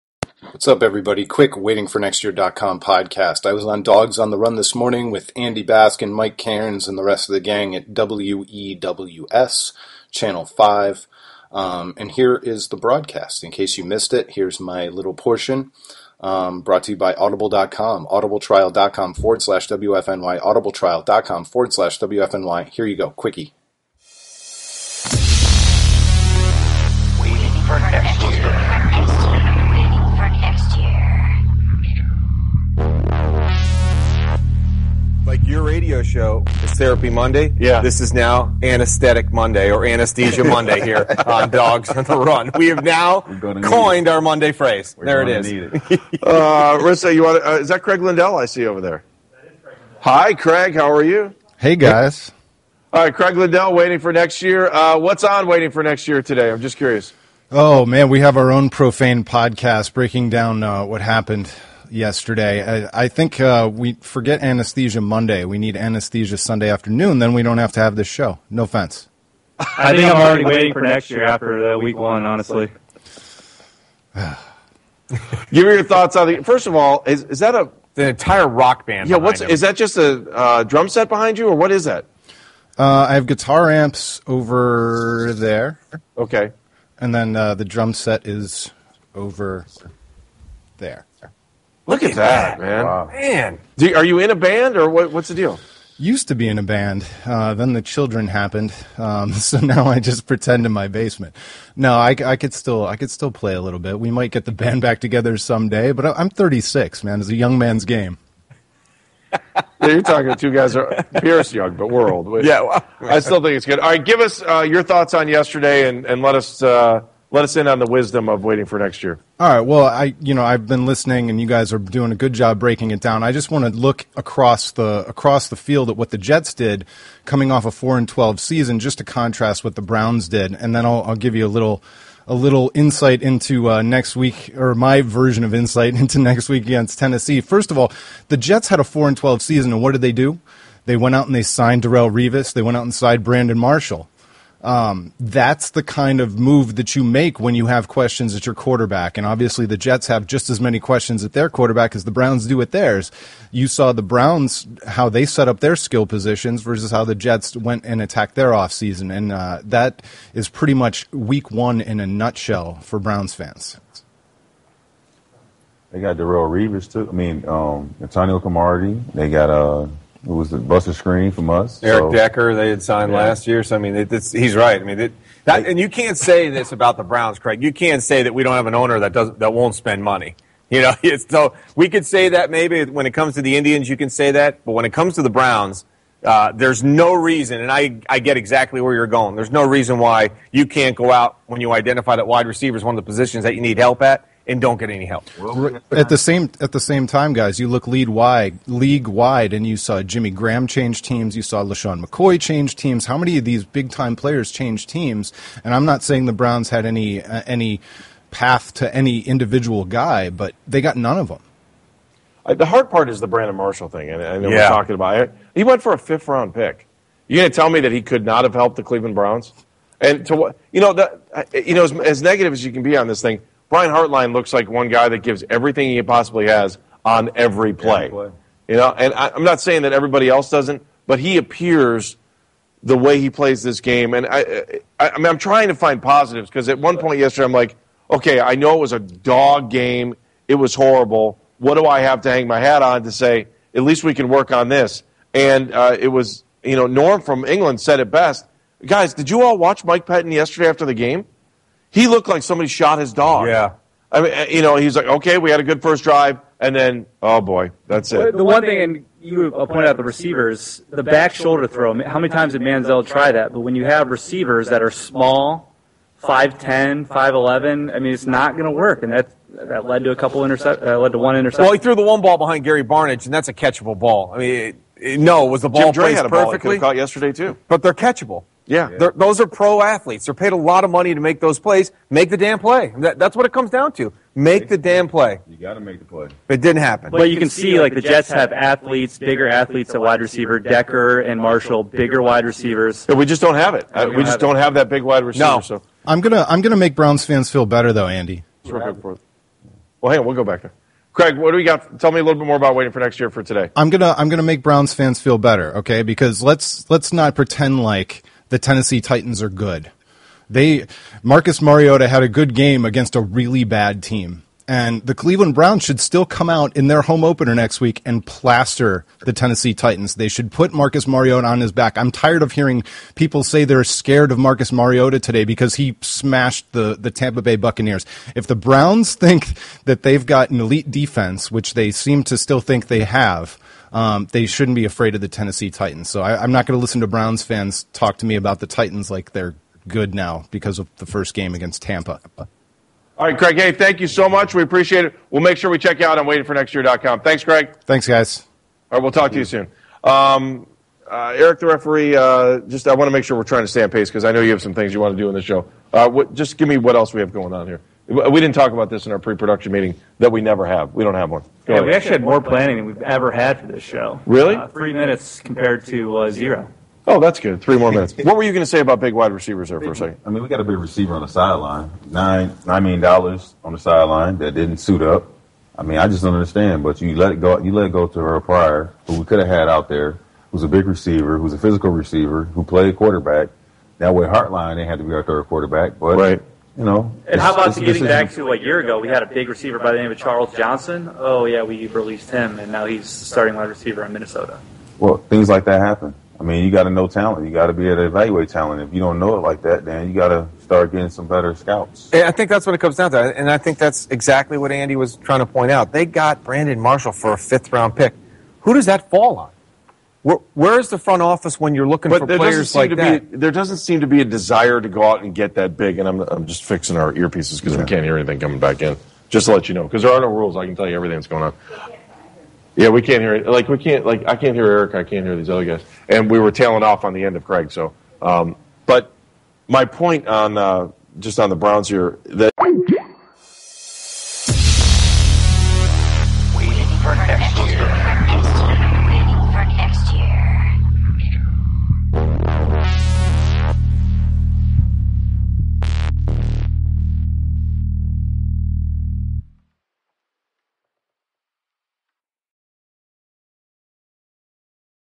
The Browns should still crush the Tennessee Titans - WFNY Podcast - 2015-09-14
I jumped on the first week of Dawgs on the Run on WEWS. It's the third year running the show and Andy Baskin and Mike Cairns had me on to discuss the week one loss to the Jets.

Please do us a favor and sign up for FanDuel with WFNY.

The Browns failed in their offseason
The Browns don't have to lose against Tennessee
Not afraid of Marcus Mariota because he beat up Tampa
The Browns didn't get Brandon Marshall or Darrelle Revis
The Browns' off-season and Jimmy Haslam's willingness to spend money